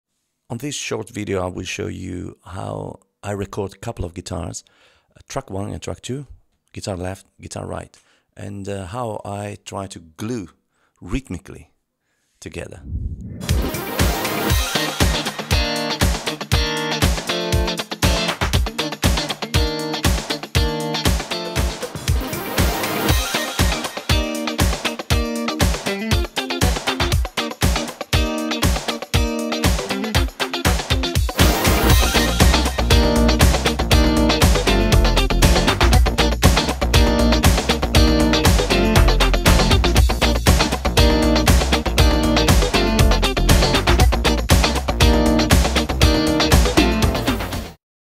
PLAYING WITH ANOTHER GUITARIST
✅USE HEADPHONES FOR WIDE STEREO IMAGE
Gtr:Left and Gtr:Right.
Here two guitar tracks, Nile Rodgers style for a remote rec session I've done for Richard Taylor and Dan Carpenter.
The stereo effect I think is very good.
There is not editing so pretty and proudly  tight .
Super clean preset Direct Input no IR.